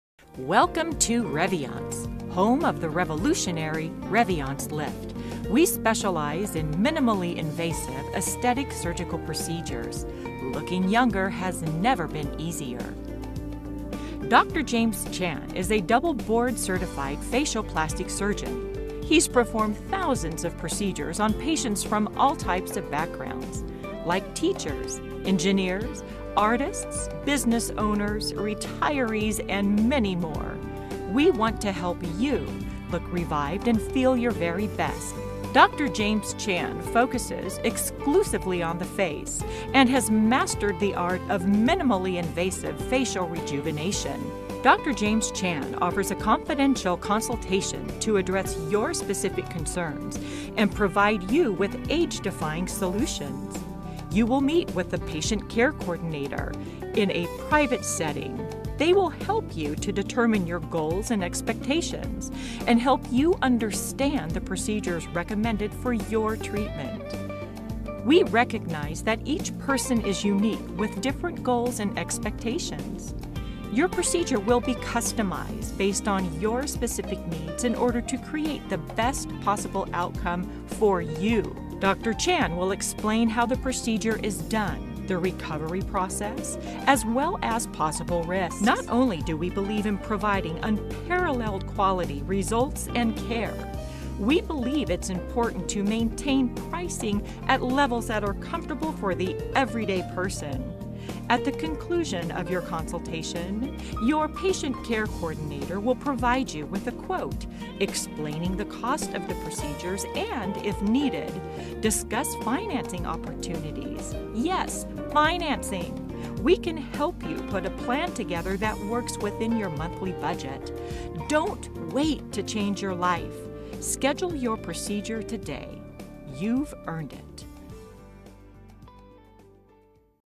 Reviance of Portland Plastic Surgery Consultation Process
Did you ever wonder what to expect at your facial plastic surgery consultation? Dr. James Chan and Yvonne Troutman, patient care coordinator, cover the ins and outs of a plastic surgery consultation at Reviance of Portland, Oregon.